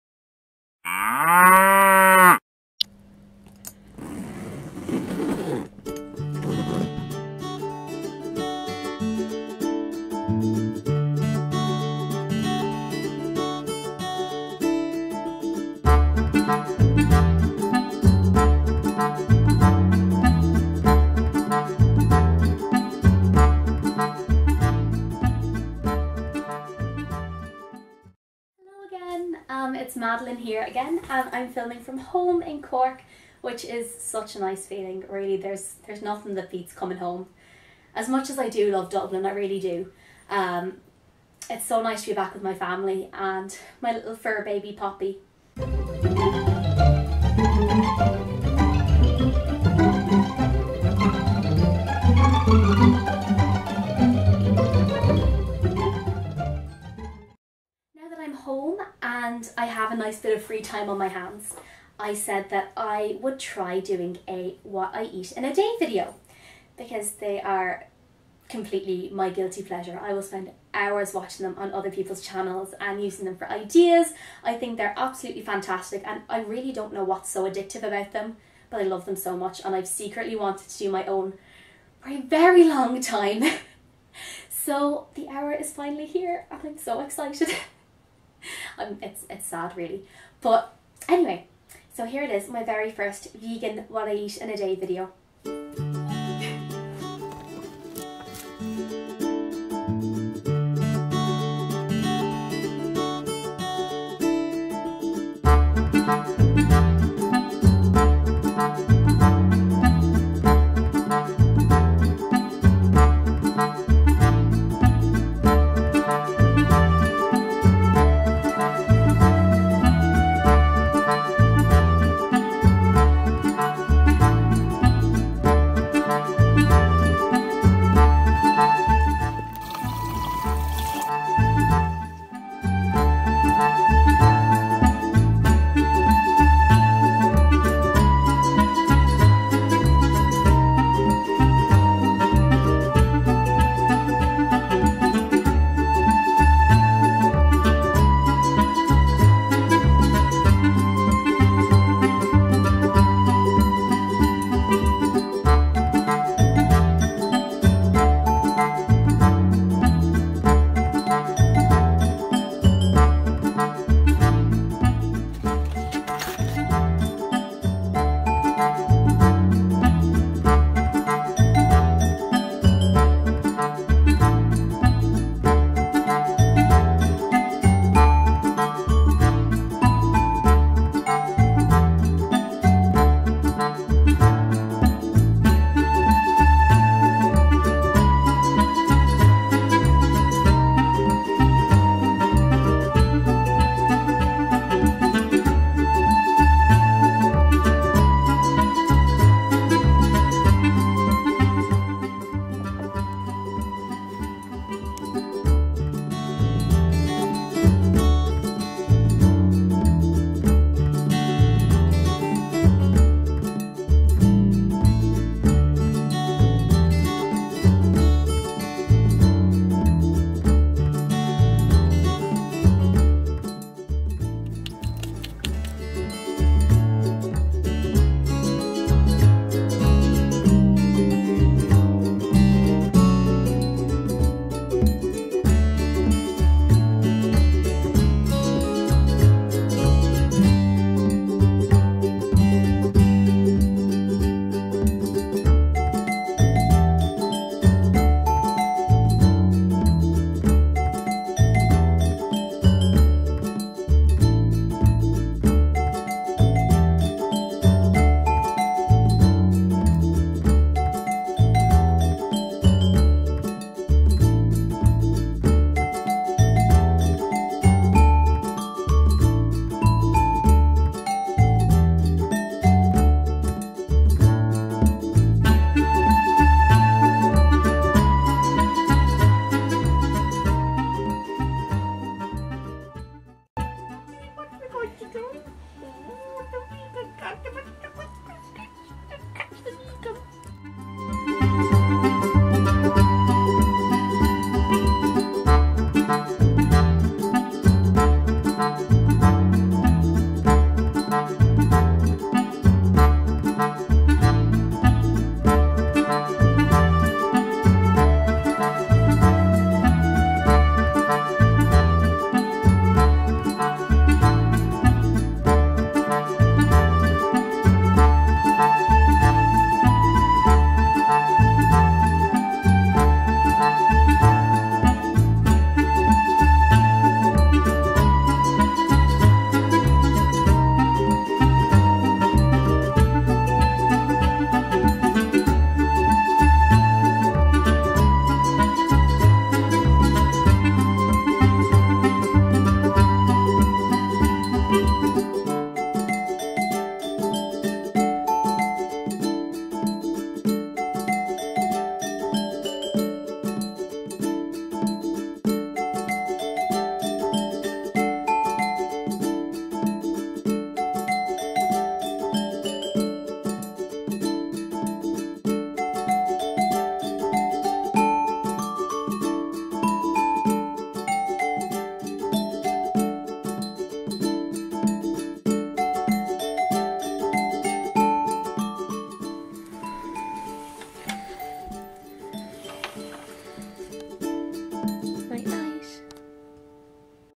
What I Eat in a Day - Vegan
After spending so much time gawking at other people's vegan What I Eat in a Day videos, I finally decided to make my own (and I had an absolute ball in the process!)

(Full credit for the sweet potato cakes recipe to Deliciously Ella!)

Here's the absolute must-watch video from Vegan Geezer that inspired me to start up this channel. Definitely take a look!